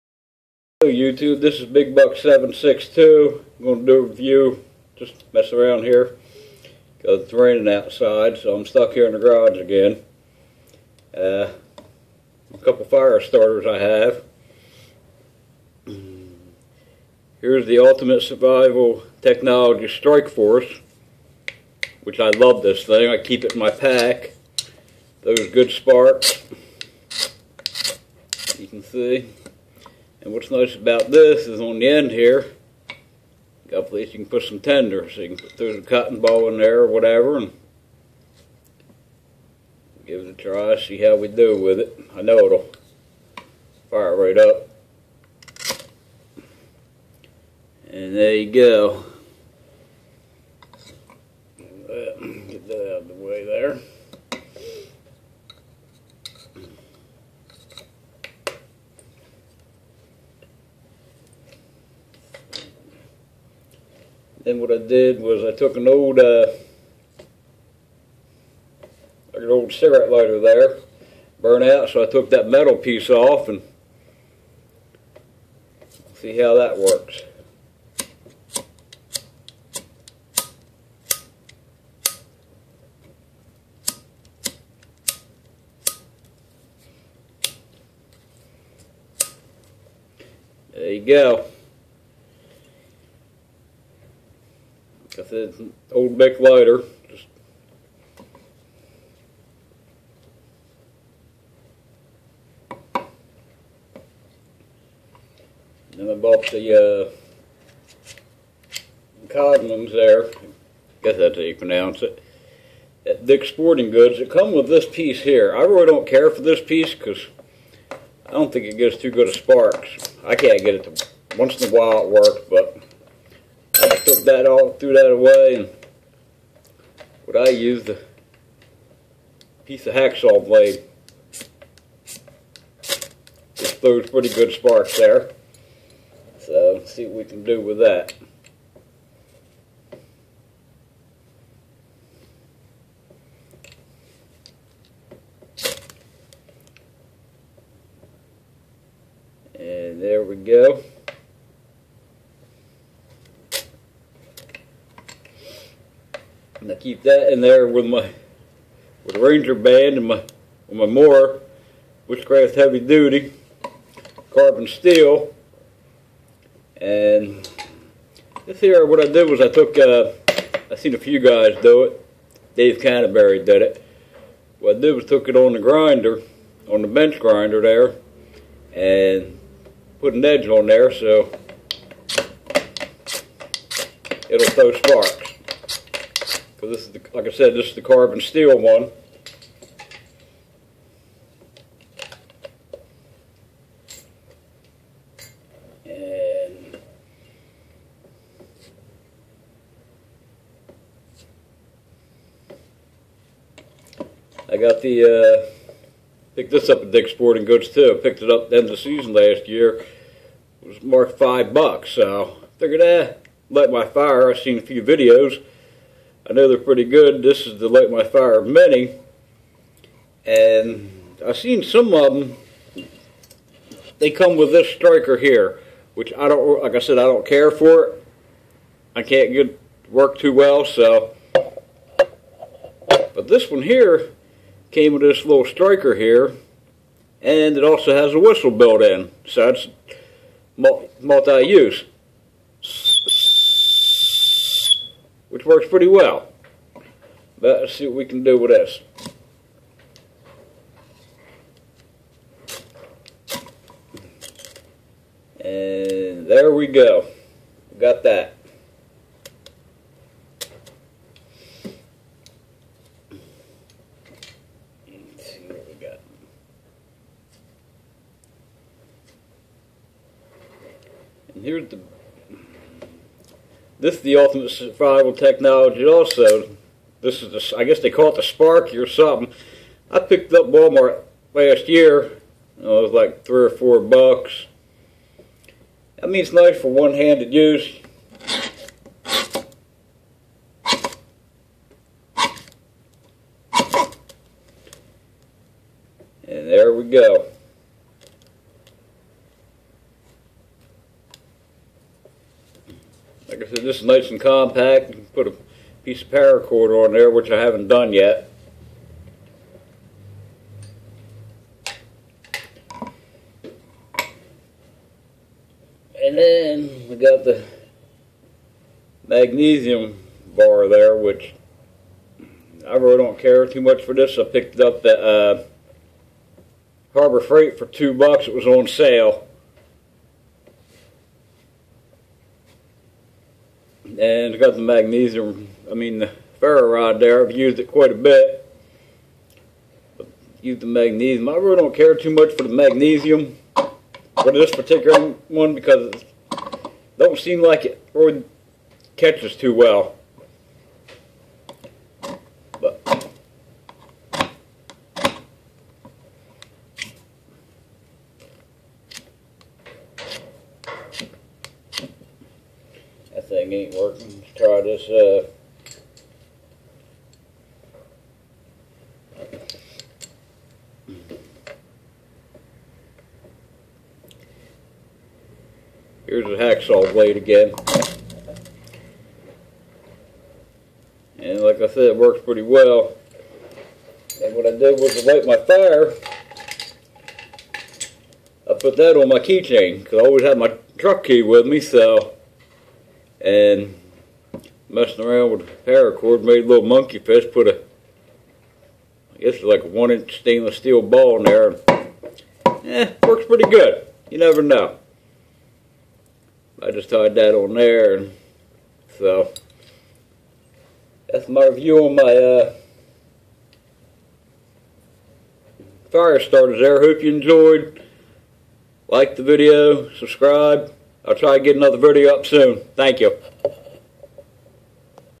My fire starters review
The fire starters that I used light my fire many ultimate survival technologies Strikeforce the Sparky a Coghlan's and an old Bic lighter